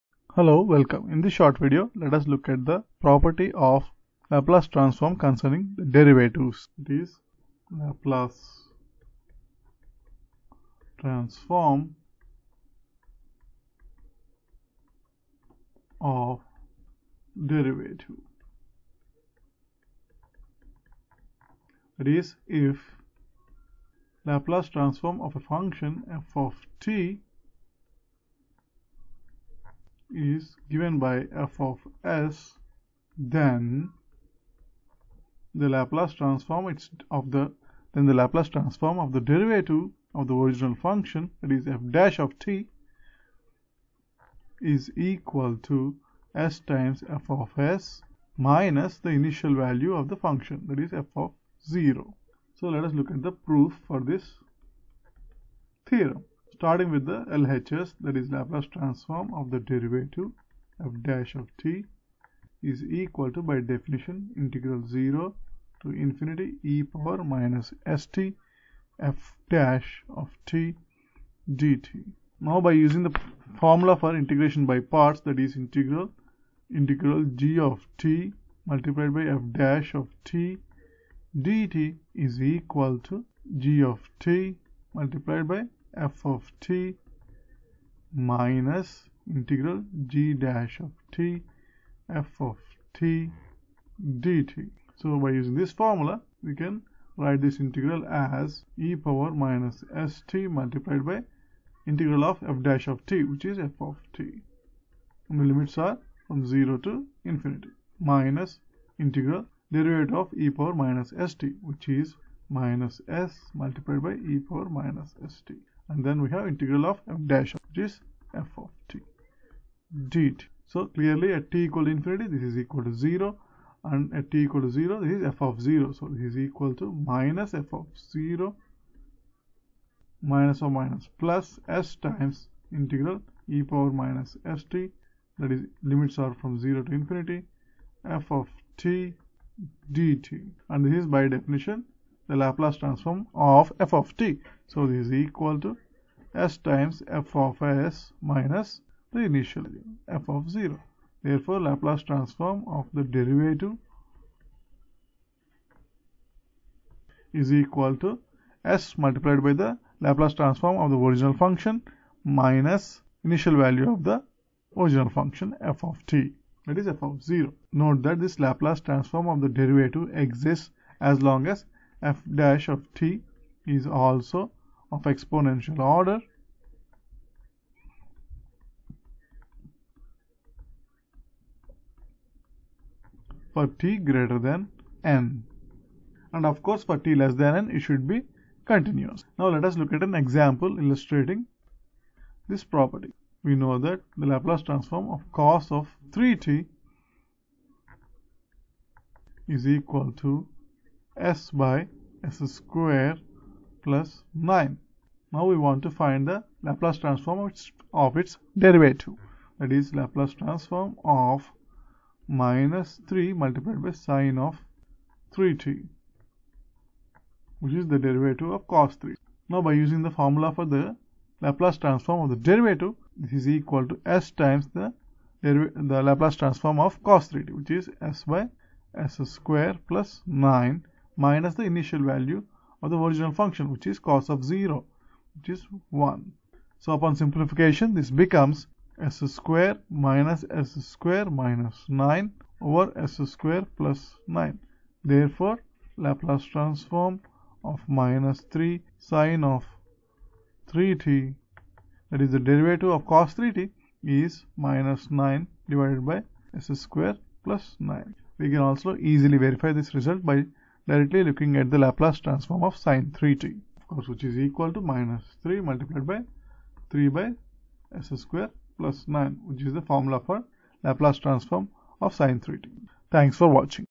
Laplace transform of derivatives
proof for the derivative property of Laplace transforms